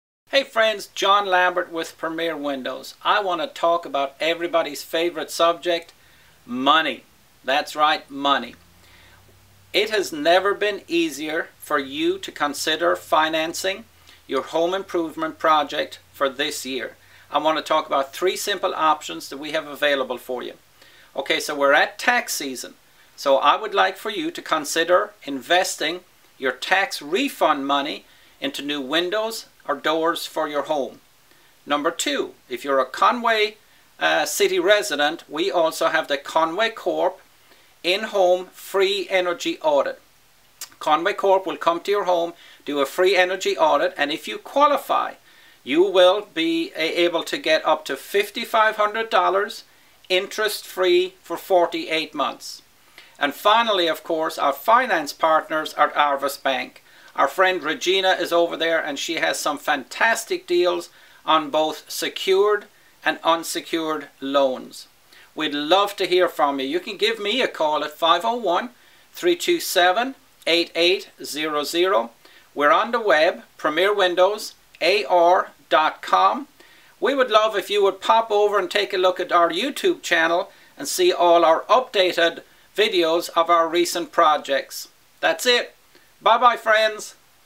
3 Simple Ways to Finance Your Home Improvement
Here are three simple ways you could finance your next home improvement!